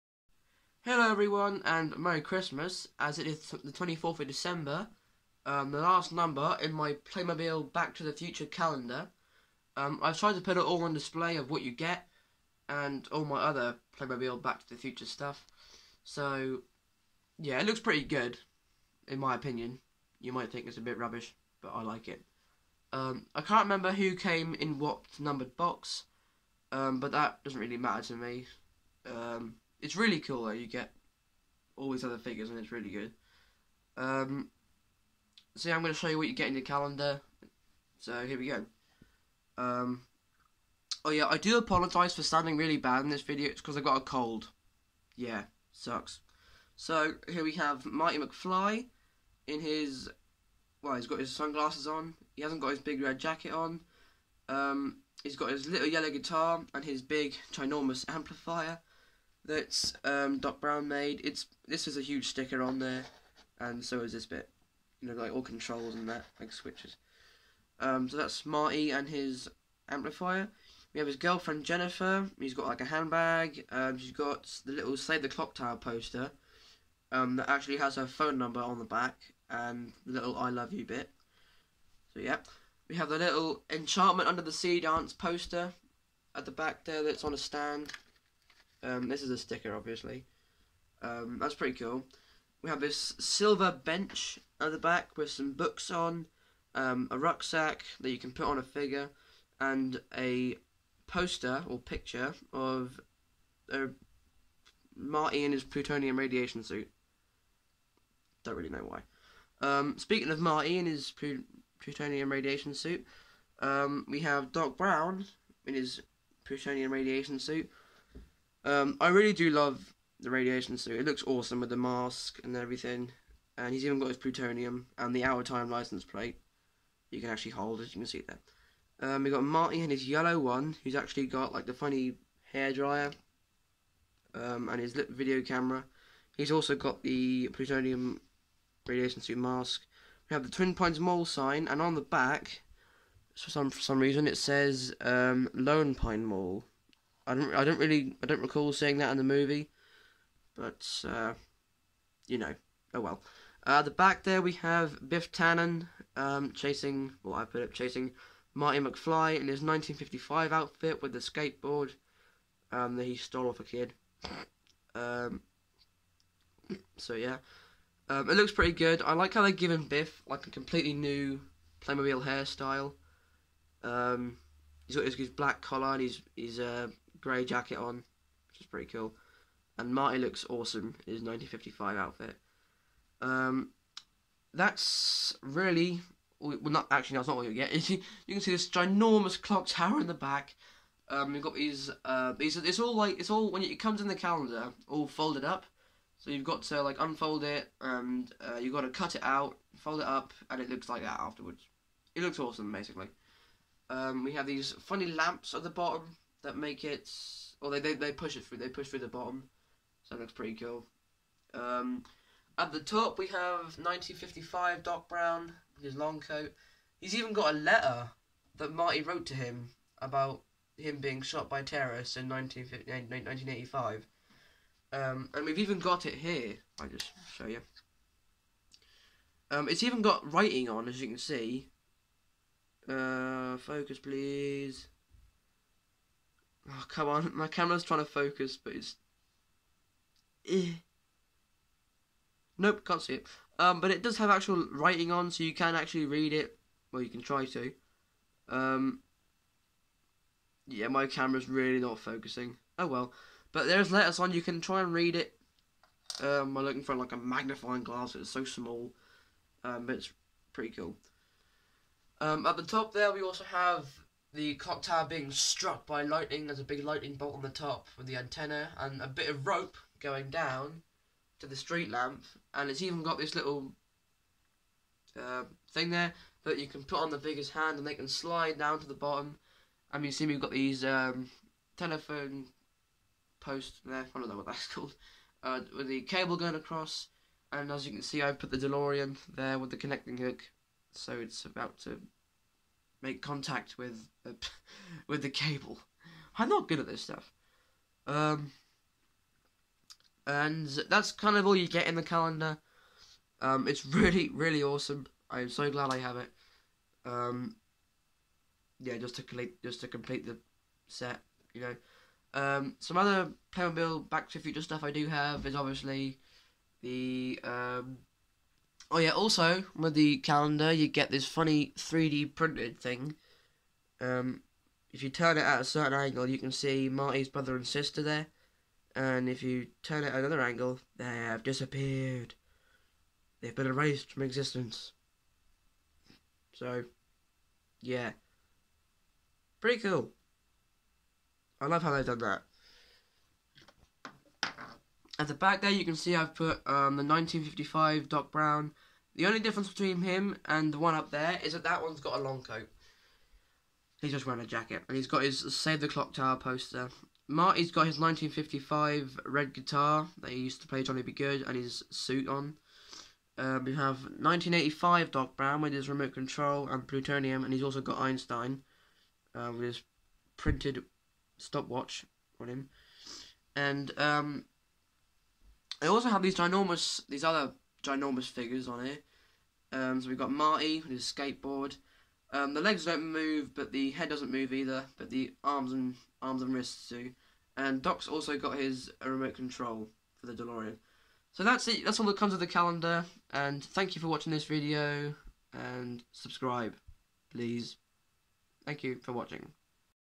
My Playmobil back to the future calender
I decided to show you all my Playmobil back to the future calender. I'm sorry if I sounded a bit funny, it's because I have a cold. As you can see I'm not very good at reviews as I haven't done one before but I hoped you enjoyed the video anyway.